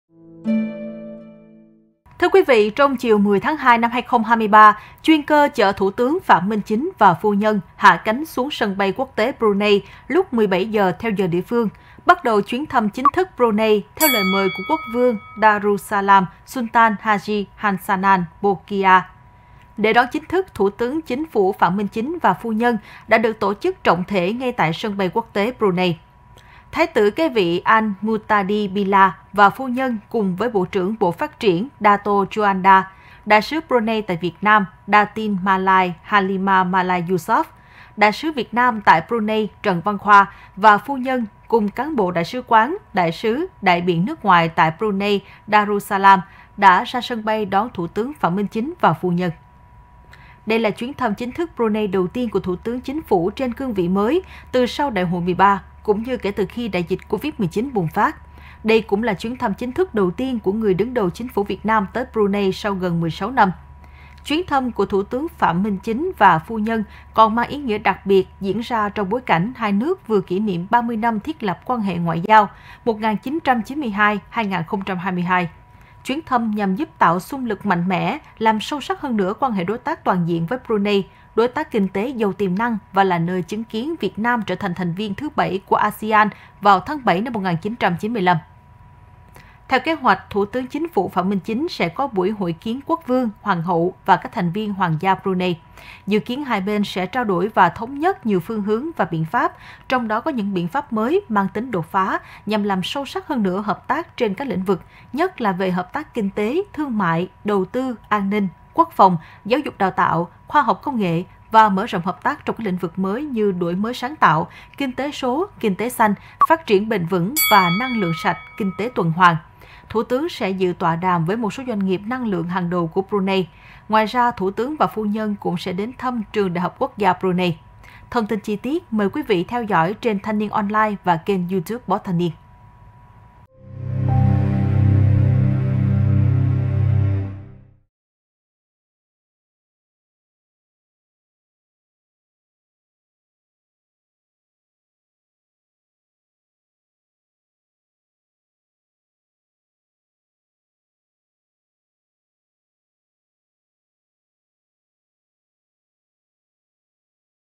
Thái tử kế vị Brunei trọng thể đón Thủ tướng Phạm Minh Chính và phu nhân
Trong chiều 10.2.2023, chuyên cơ chở Thủ tướng Phạm Minh Chính và phu nhân hạ cánh xuống sân bay quốc tế Brunei, lúc 17 giờ (giờ địa phương), bắt đầu chuyến thăm chính thức Brunei theo lời mời của Quốc vương Darussalam Sultan Haji Hassanal Bolkiah.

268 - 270 Nguyễn Đình Chiểu, Phường Võ Thị Sáu, Q.3, TP HCM